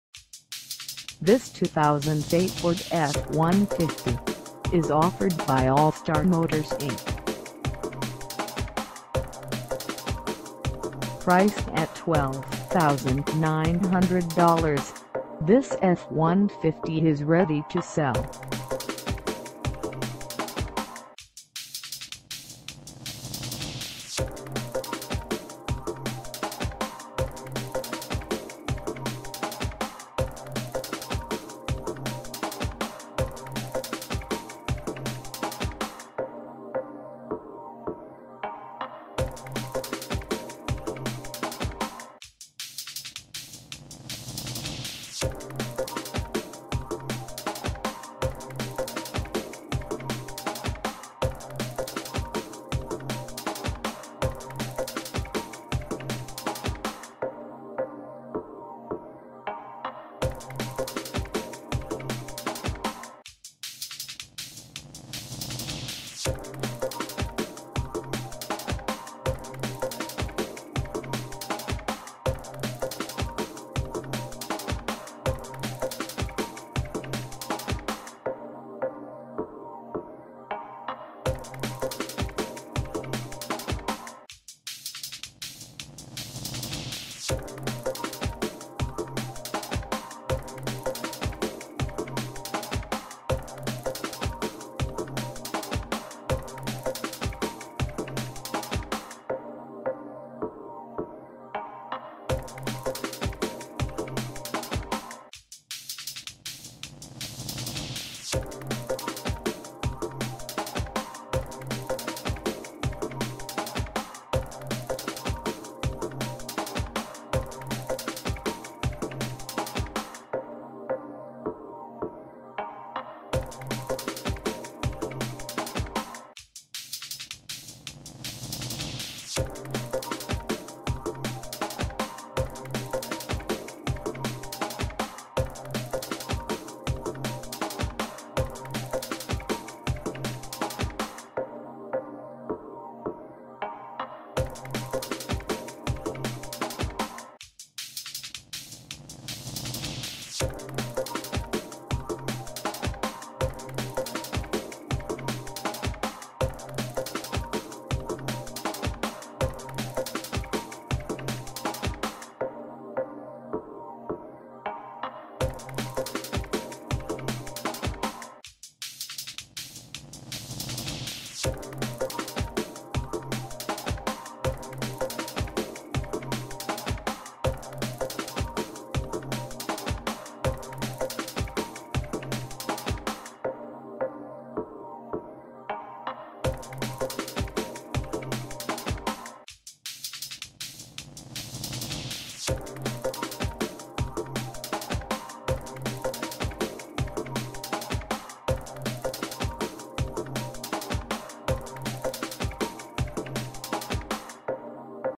2008 Ford F150 XL 2WD - for sale in MIDDLEBURG, FL 32068
ALLSTAR MOTORS INC
2739 BLANDING BLVD

Come test dirve this 2008 Ford F150 XL 2WD for sale in MIDDLEBURG, FL.

allstarmotorsinc.net Carfax Certified check out the free report here !! This 2008 Ford F150 has a V6 automastic ac power steering power brakes am/fm cd alloy wheels bedliner and as always the nicest one at the best price.

|2 Door|Short Box|2 Wheel Drive|Automatic Transmission|Alloy Wheels|Bedliner|Receiver Hitch|Towing Package|Air Conditioning|Cloth Seats|Power Steering|Vinyl Floor|Alarm|CD Player|Driver Air Bag|Passenger Air Bag|